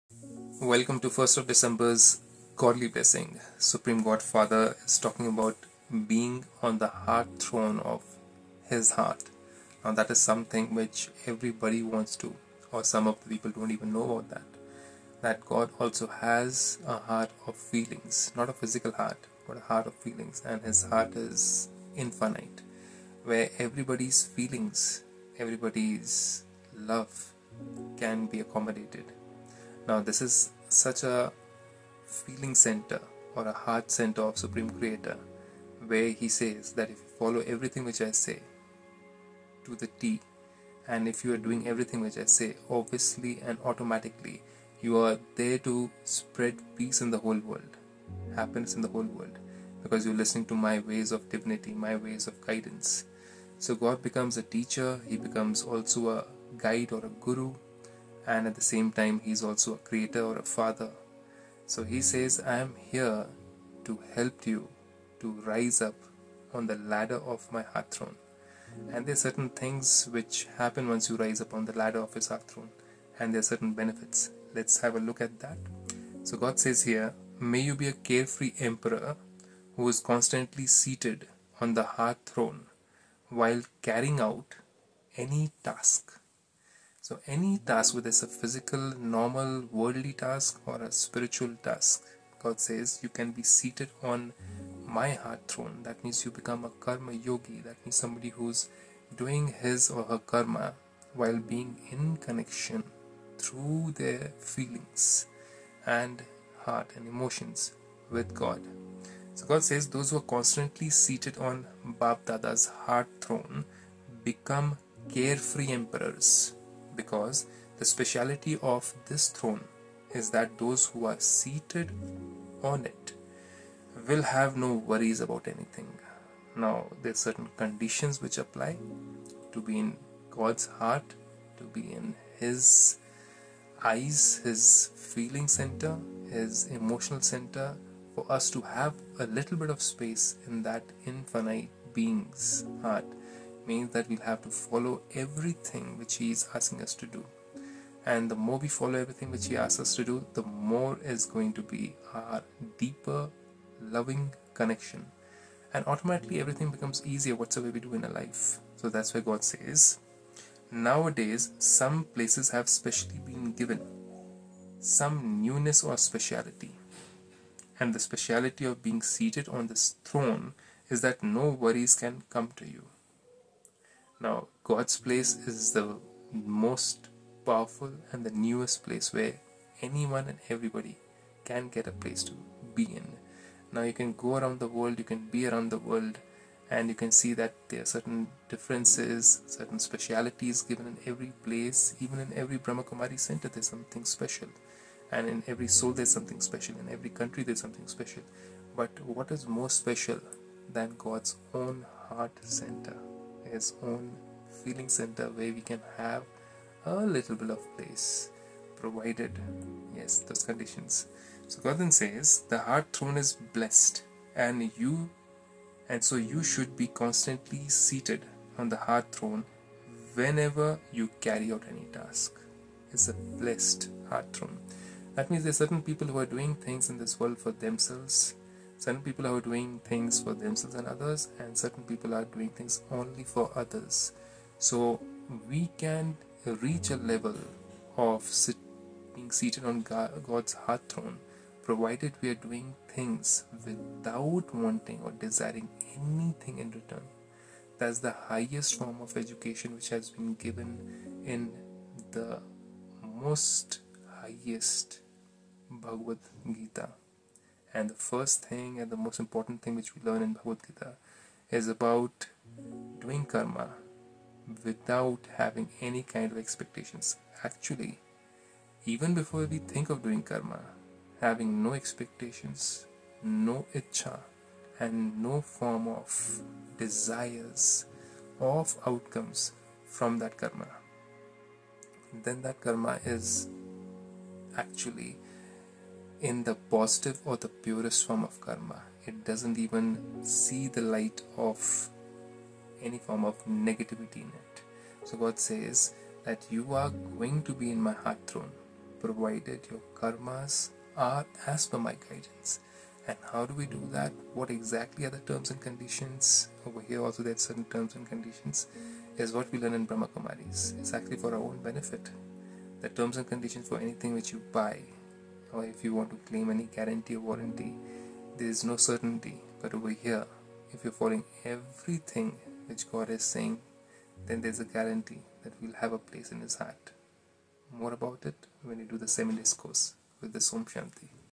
Be a carefree emperor who is constantly seated on the heart throne while carrying out any task.
Day 304 May you be a carefree emperor who is constantly seated on the heart throne while carrying out any task.

Om Shanti .